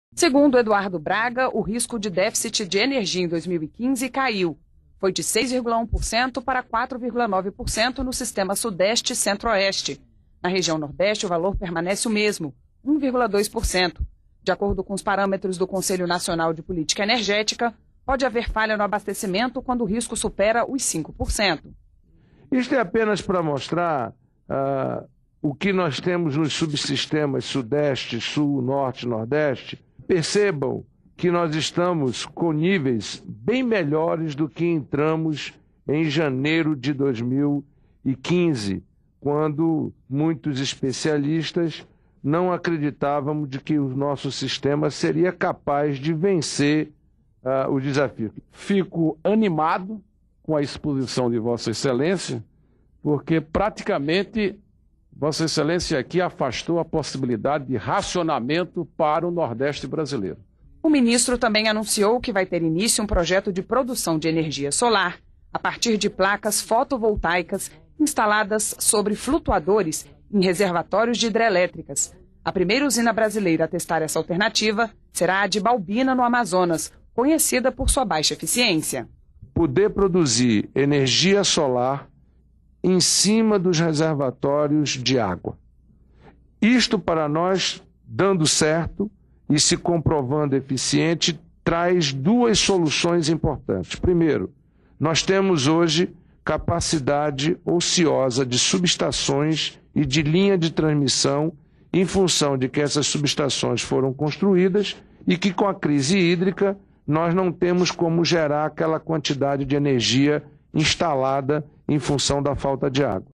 Risco de racionamento de energia cai, diz Eduardo Braga
O ministro de Minas e Energia, Eduardo Braga, minimizou o risco de racionamento de energia neste ano. Segundo ele, a capacidade dos reservatórios está aumentando, e o governo trabalha com a possibilidade de ampliação de fontes alternativas de energia, como a solar. Braga participou de uma audiência na Comissão de Infraestrutura, como mostra a reportagem da TV Senado.